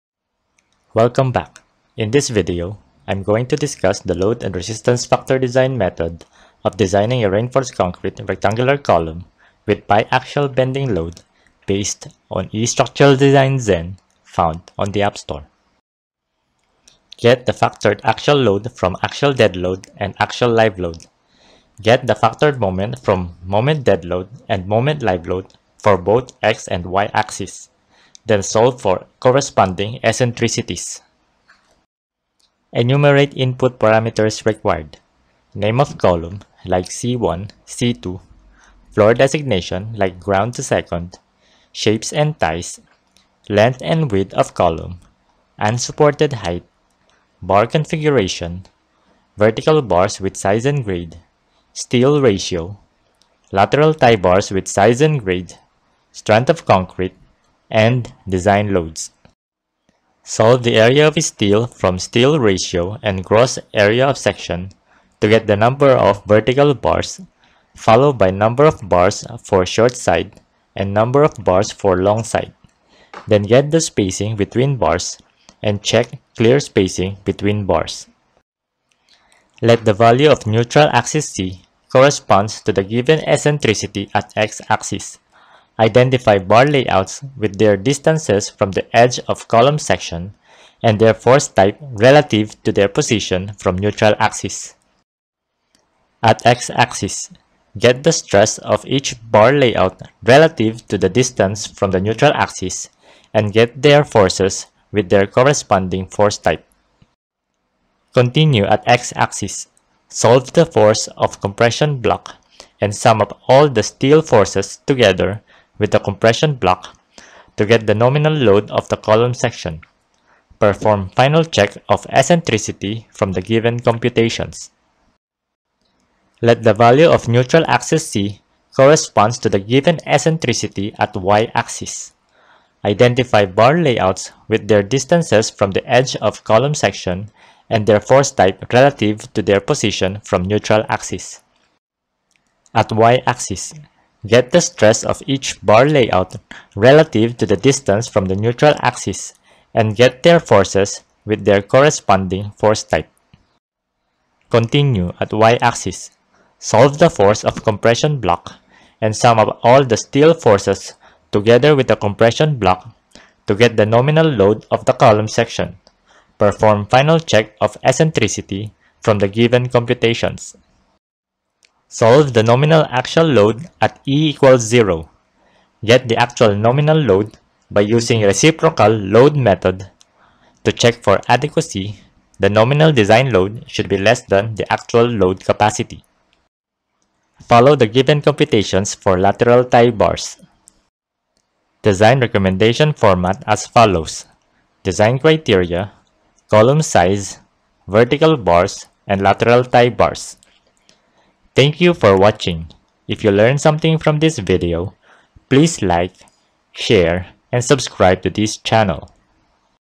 How To Design a Rectangular Column with Biaxial Bending Using LRFD -Start Learning Structural Design
This video discusses step by step Formula of solving reinforced concrete rectangular column with biaxial bending load using Load and Resistance Factor Design based on eStructural Design Zen found on the App Store.